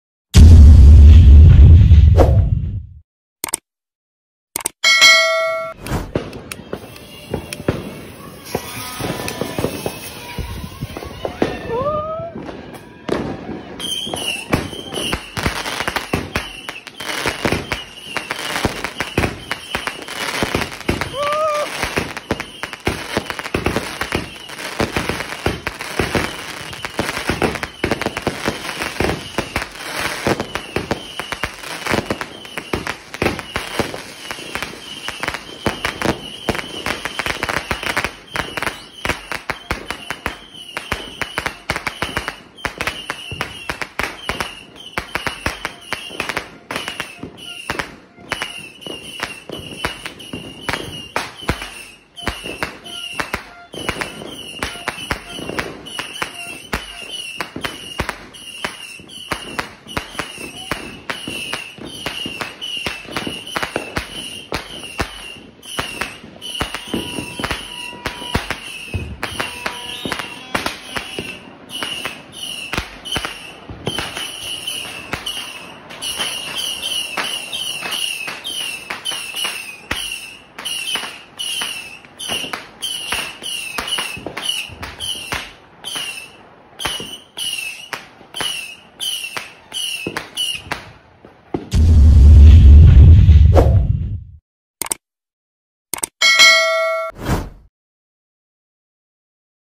Dragon’s Sword of Valor 16 shots by Dragon Fireworks + 300 shots Missile | New Year 2023 Philippines
Watch my Previous Video:
Dragon Lance by Dragon Fireworks | New Year 2023 Philippines